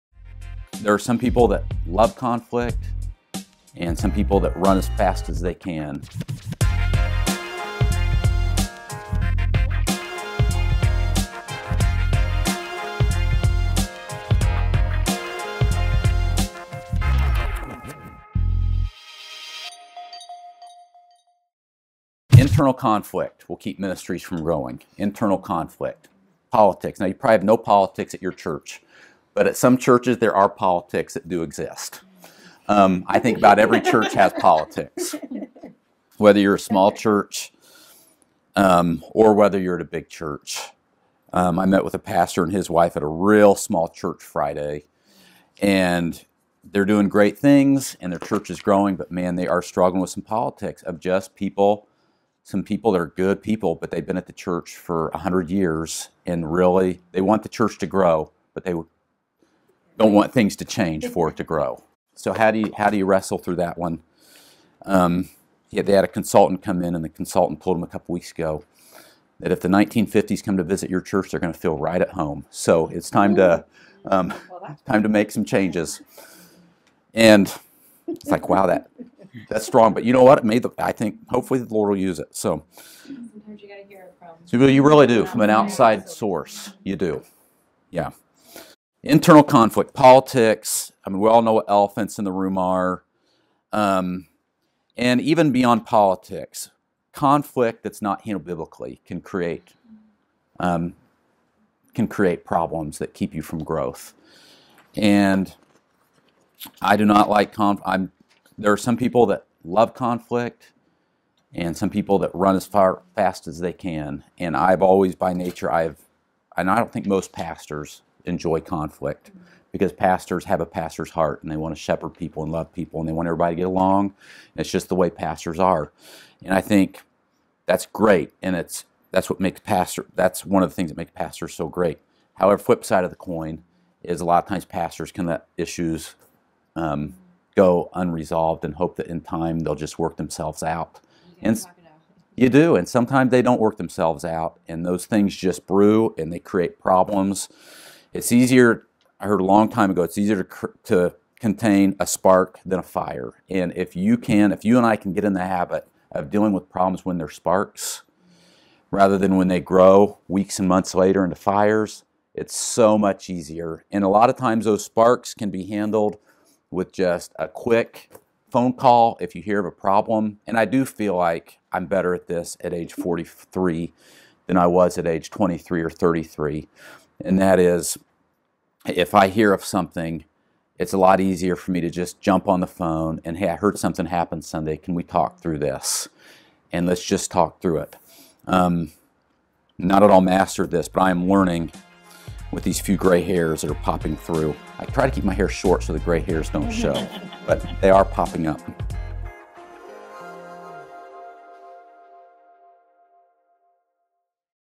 Contain the Spark Before It's a Flame
Conflicts, politics, the elephant in the room. Not usually pleasant things to deal with in your organization, but they must be dealt with and the earlier the better. Today Ryan Frank discusses the importance of keeping a spark from becoming a flame.